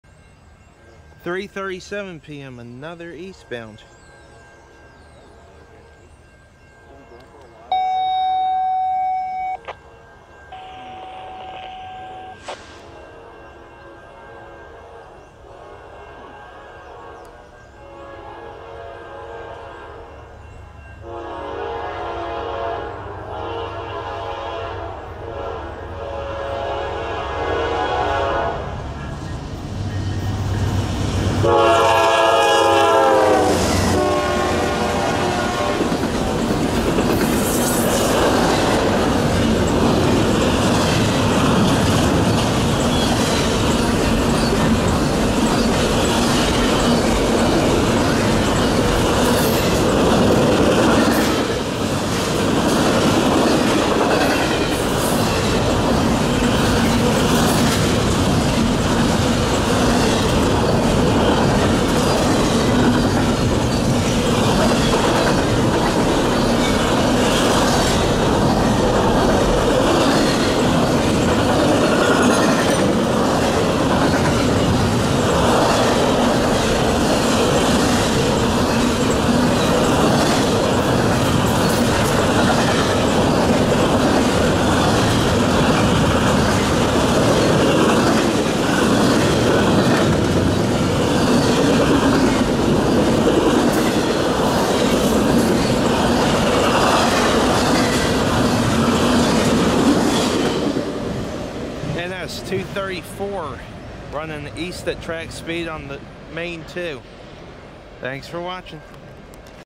NS 234
3:37 PM

Next up was NS 234 at full track speed on track 2. By this point a lot of the trains had run already which meant we'd be in for a slow night! Shot in 4K with a Sony Handycam FDR-AX53.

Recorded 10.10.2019